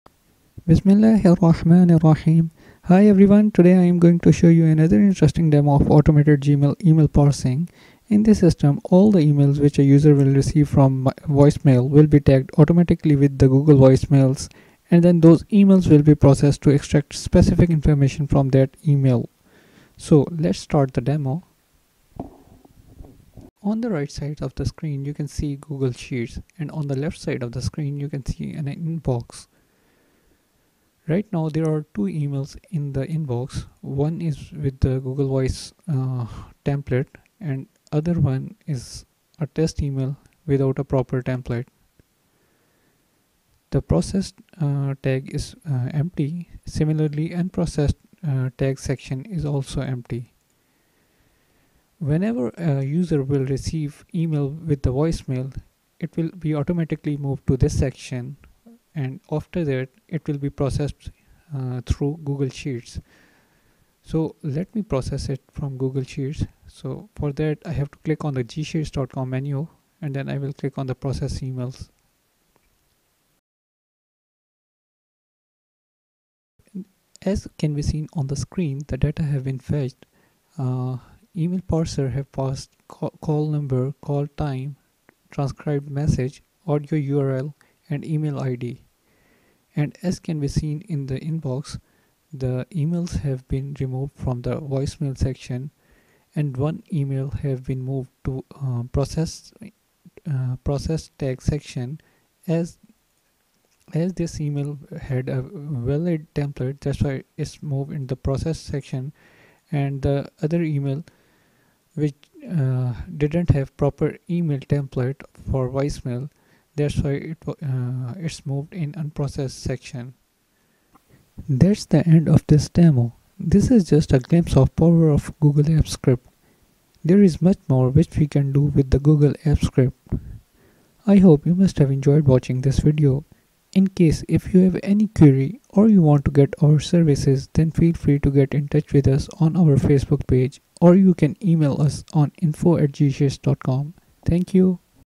Gmail Parsing To Google Sheets Using Google Apps Script [Demo]
Automated Gmail
Email Parsing To
Google Sheets Using
Google Apps Script

Note: This is a demo video and NOT a tutorial.